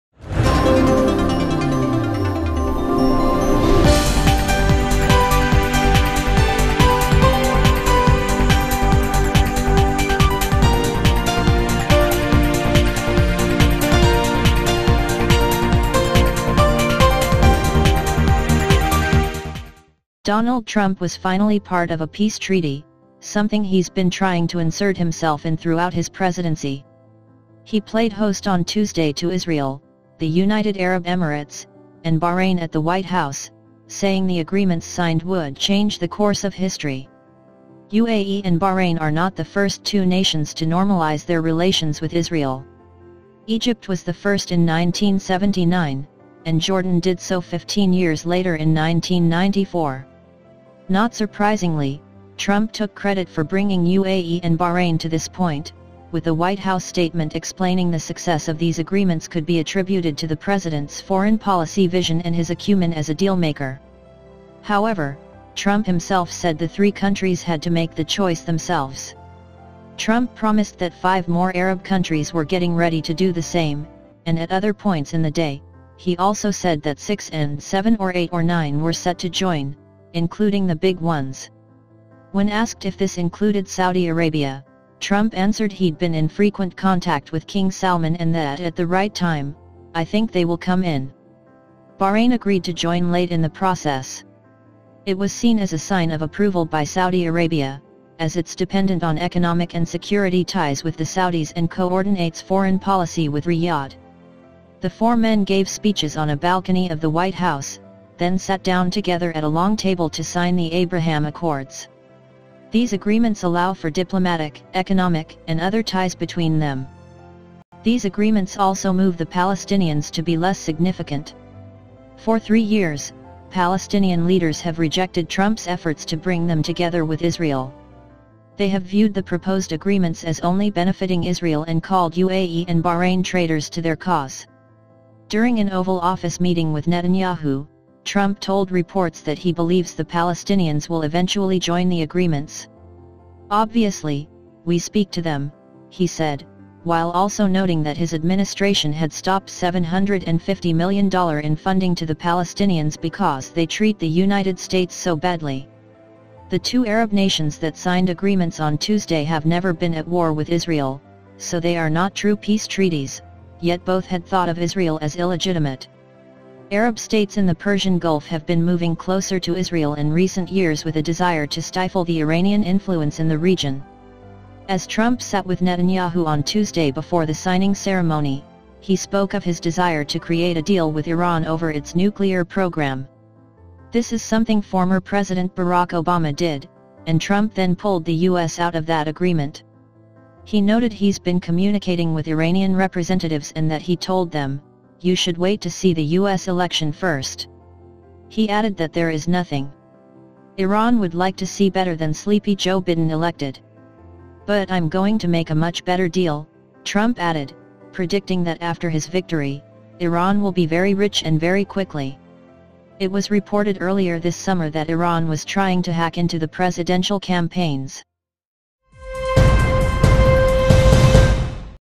Israel, UAE, Bahrain Sign Historic Deal at White House
Donald Trump was finally part of a peace treaty, something he's been trying to insert himself in throughout his presidency. He played host on Tuesday to Israel, the United Arab Emirates, and Bahrain at the White House, saying the agreements signed would "change the course of history."

UAE and Bahrain are not the first two nations to normalize their relations with Israel. Egypt was the first in 1979, and Jordan did so 15 years later in 1994. Not surprisingly, Trump took credit for bringing UAE and Bahrain to this point, with a White House statement explaining the success of these agreements could be attributed to the president's "foreign policy vision and his acumen as a dealmaker." However, Trump himself said the three countries "had to make the choice themselves